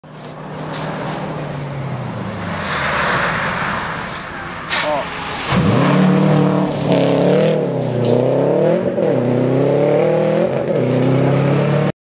2010 Rally America 100 Acre Wood stage 2.5 video 9
Stage 2, AKA: Pandora Westover 1 of the 2010 100 Acre wood rally, taken at marshal point 5.